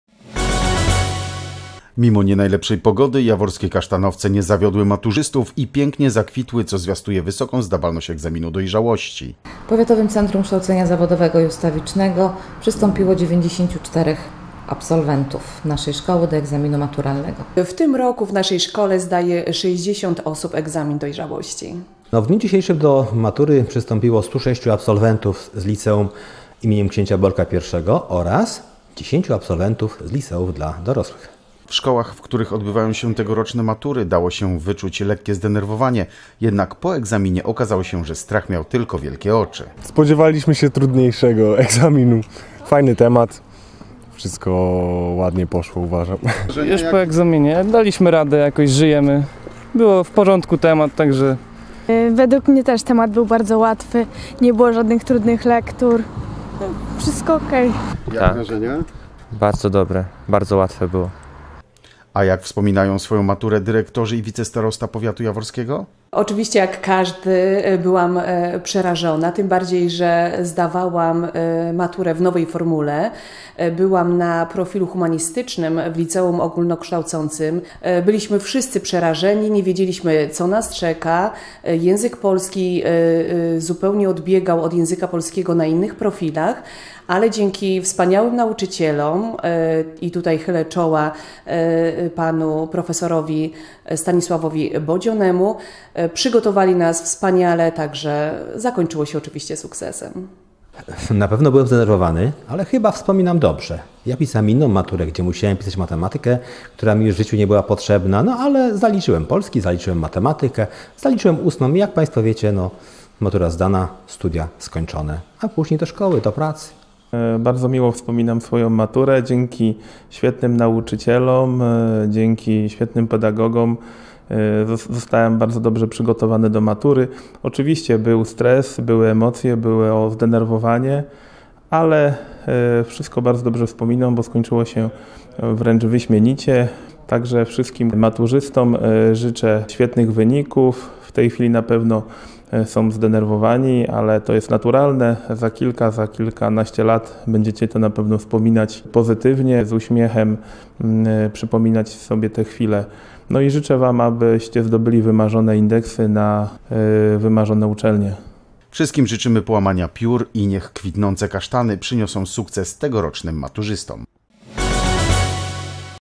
100 dni temu Polonez, teraz czas matur.
Mimo nie najlepszej pogody jaworskie kasztanowce nie zawiodły maturzystów i pięknie zakwitły co zwiastuje wysoką zdawalność egzaminu dojrzałości. W szkołach , w których odbywają się tegoroczne matury dało się wyczuć lekkie zdenerwowanie, jednak po egzaminie okazało się że strach miał tylko wielkie oczy.
A jak wspominają  swoją maturę dyrektorzy i wicestarosta powiatu jaworskiego?
Wszystkim życzymy połamania piór i niech kwitnące kasztany przyniosą sukces tegorocznym maturzystom.